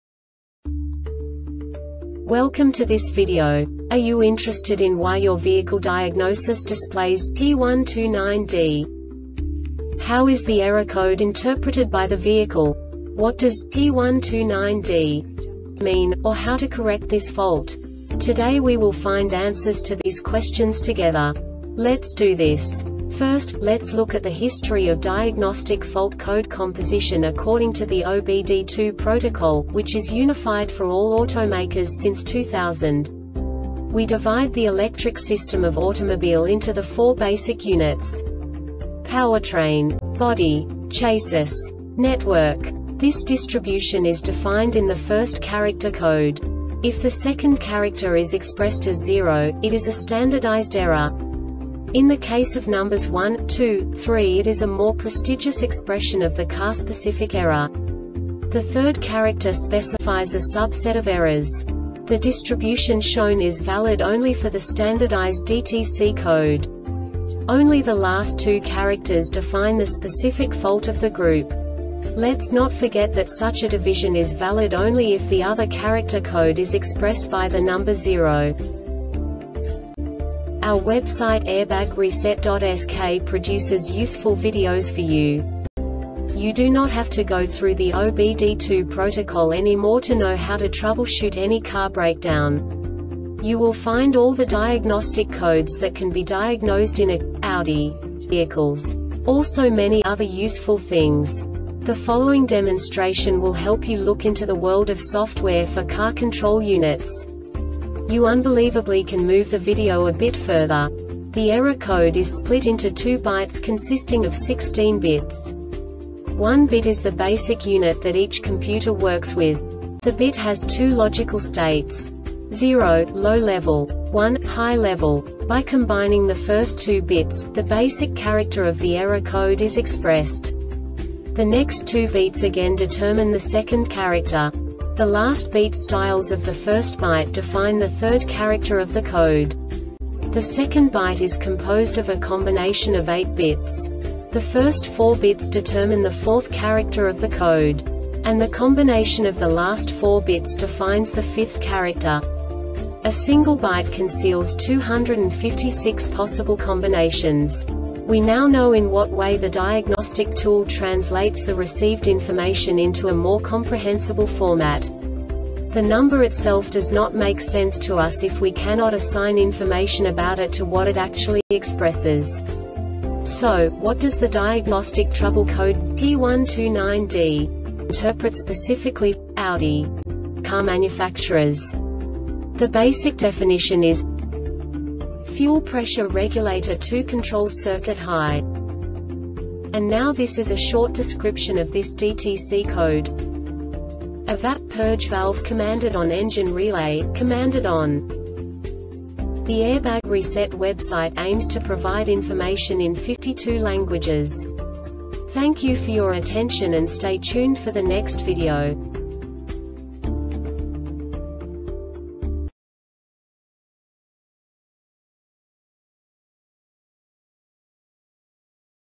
DTC Audi P129D Short Explanation
Contents:
0:21 Basic DTC analysis according to OBD2 protocol standard.
1:48 Insight into programming
2:58 A diagnostically understandable description of the Diagnostic Trouble Code
3:05 Basic error definition

Make:
Audi
Code:
P129D
Definition:
Fuel Pressure Regulator 2 Control Circuit High
Description:
Evap purge valve Commanded on Engine relay, Commanded on